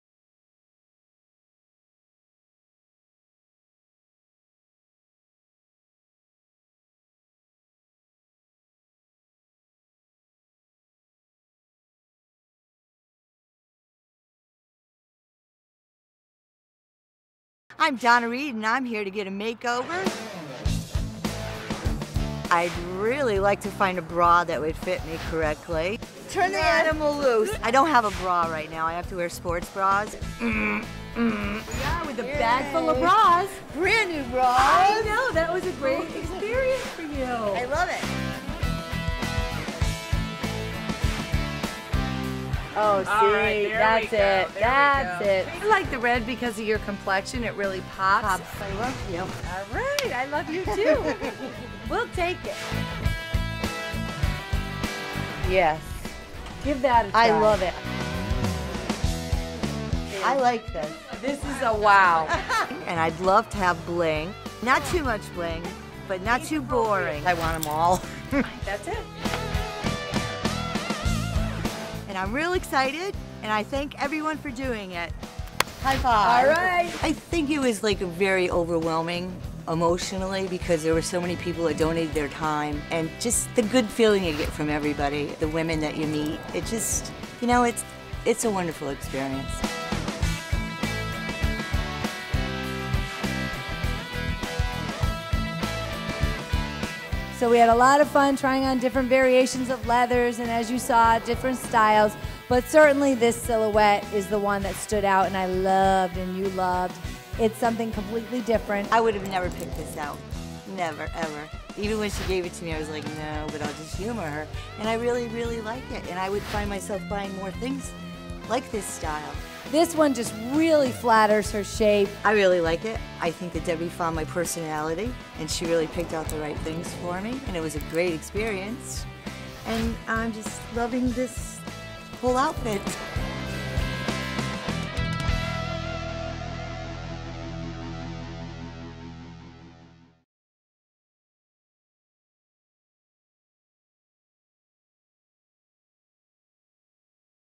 Putting a New Face on Breast Cancer - Donna | Middlesex Hospital
Donna Read, breast cancer survivor, gets a fashion makeover with the help Debbie Wright. Debbie developed and operates Project Closet, a confidence-building, fashion-styling business. The makeovers were done in conjunction with Middlesex Hospital's Annual Breast Health Event.